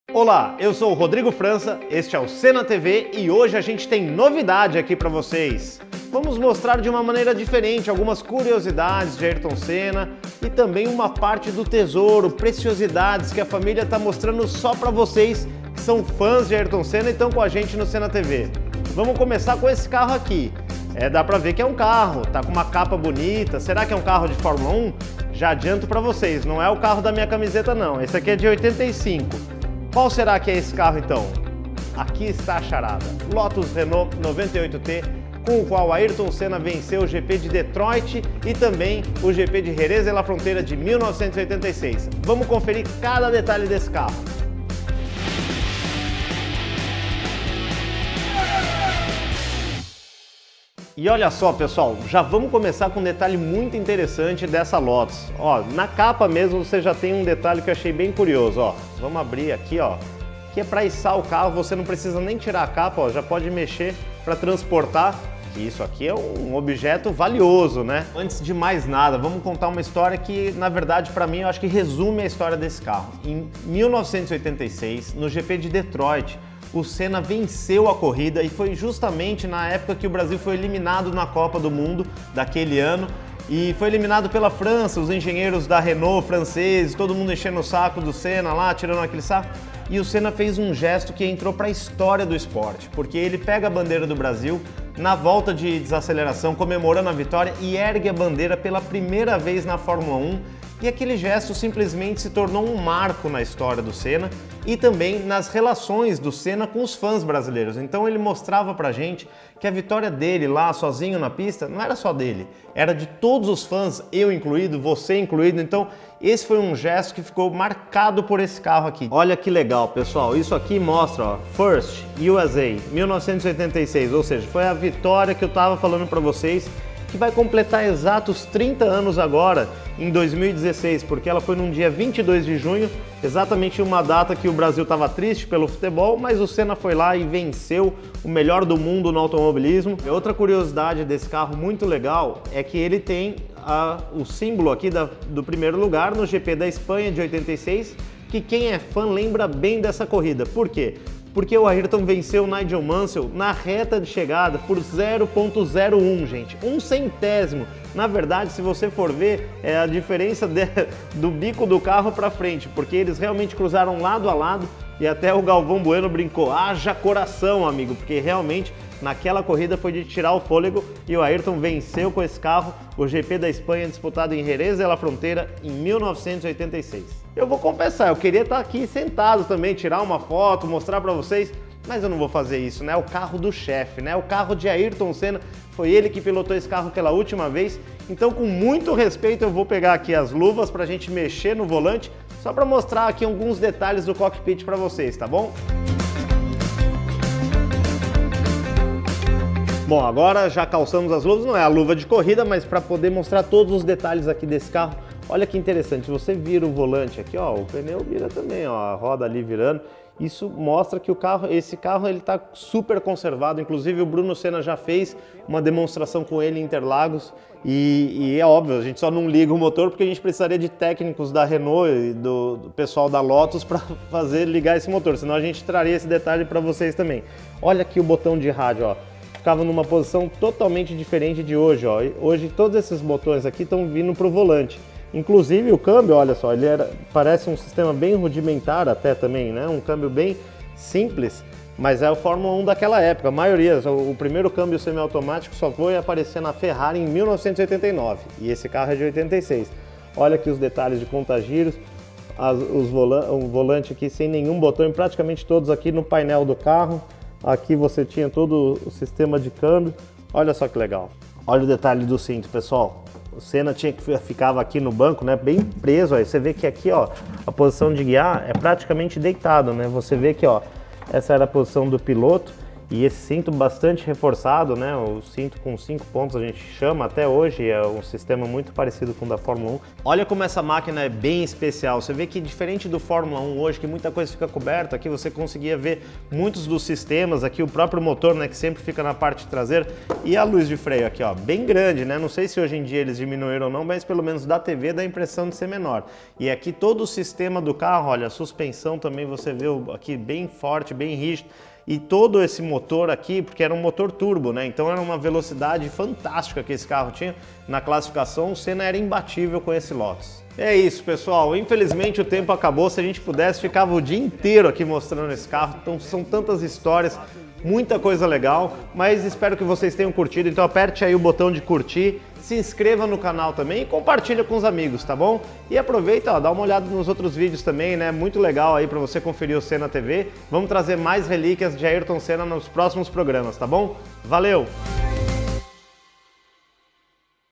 Lotus 98T | Relíquias de Ayrton Senna | SennaTV - 001
O novo quadro do SennaTV com as relíquias de Ayrton Senna estreia hoje com o pé no acelerador. Confira todos os detalhes da Lotus de 1986 pilotada pelo brasileiro no GP de Detroit, quando ele levantou a bandeira do Brasil pela primeira vez dentro do seu carro.

Apresentação: Rodrigo França
Direção e Produção: Jonatas Fróes
Captação e Edição: Maíra Villamarin
Pesquisa: Luiz Felipe Chaguri
Tradução: Bruno Tapajós
Vinheta: Caio Nery Filho
Trilha Sonora: Fábio Moraes
Apoio: Instituto Ayrton Senna

Todos os produtos que aparecem nesse vídeo estão disponíveis para venda na Ayrton Senna Shop. Visite